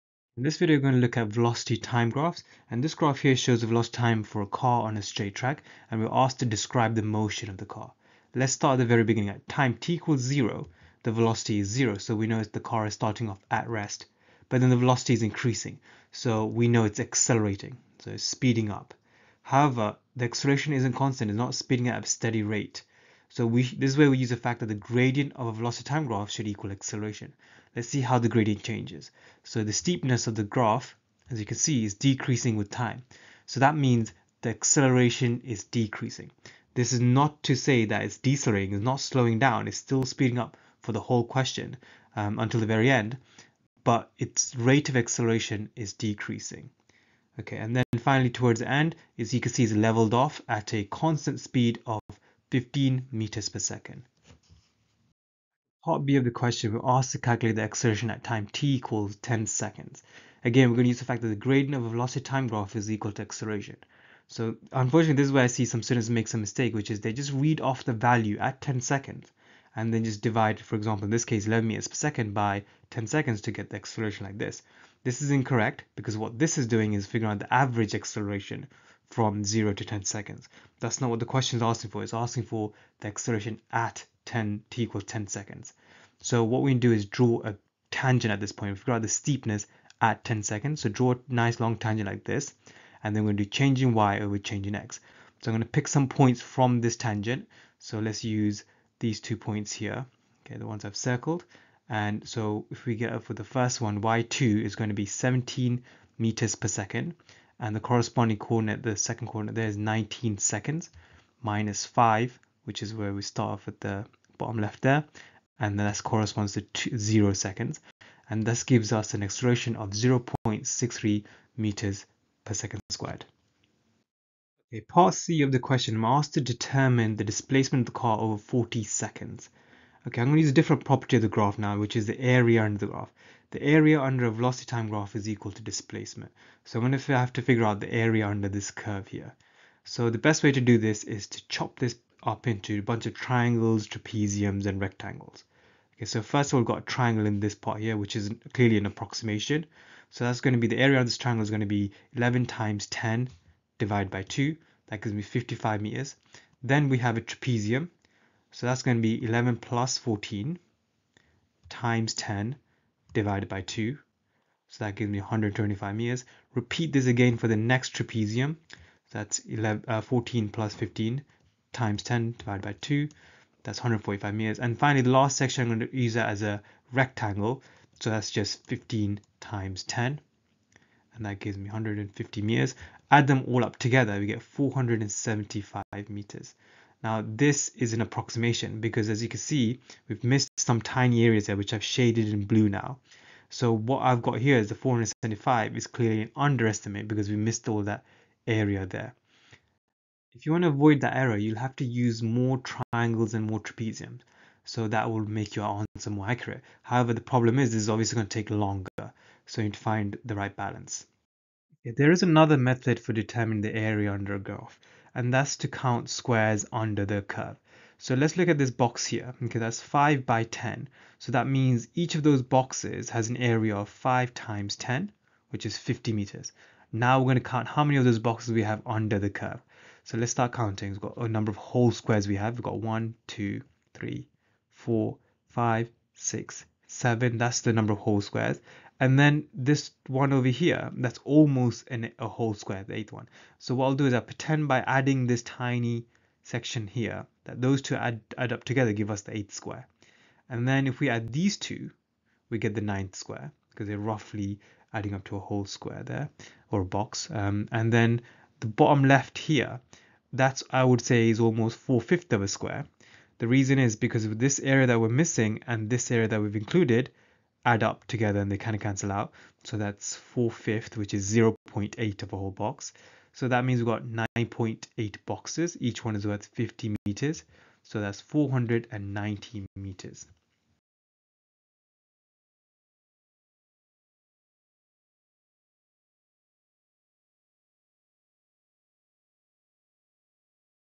1.06 What if it's non uniform acceleration?
Topics for A-level Physics: All Examboards
00:00 Basic description
00:54 Gradient / Tangent
02:03 Area using trapeziums
03:30 Area counting squares
04:40 Questions
04:43 Questions help
04:45 Answers